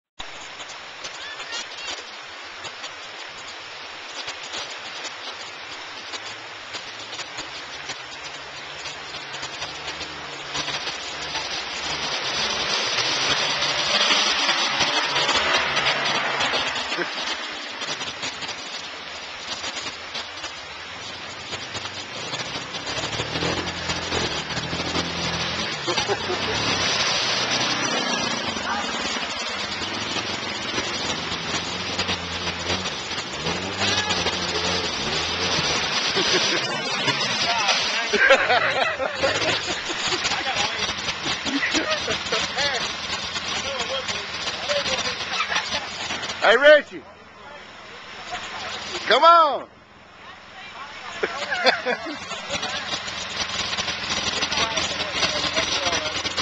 Yamaha Rhino Polaris Ranger RZR S Crossing Elkhorn Creek
Crossing Elkhorn Creek Sunday May 10th In a Rhino, Polaris Rzr  S and Ranger Zip code 41520 come ride